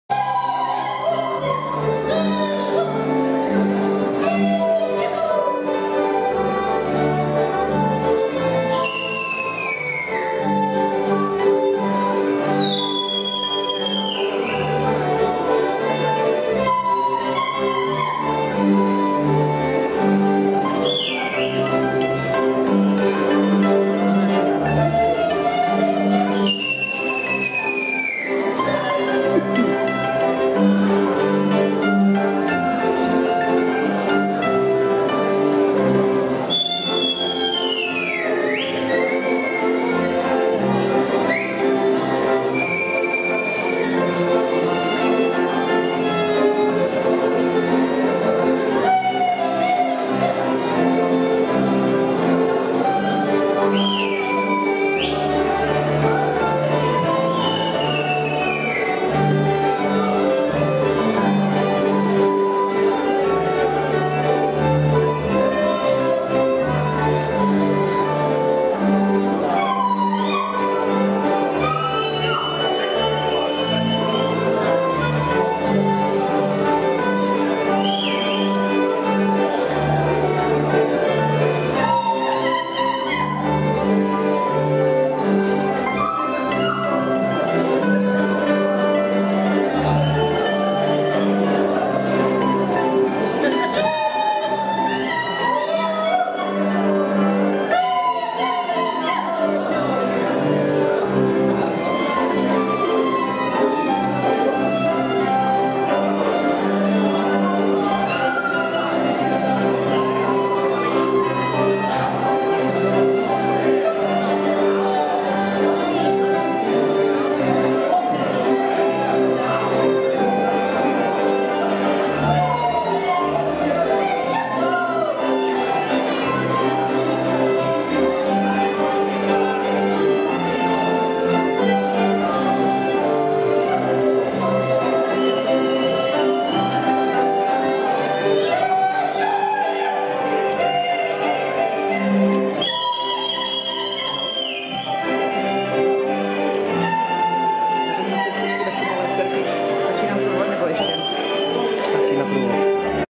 Valašský bál souborů Portáš a Vizovjánek ve Vizovicích 2012
Slavnostní nástup krojovaných na Valašském bále souborů Portáš a Vizovjánek v sokolovně Vizovice 2012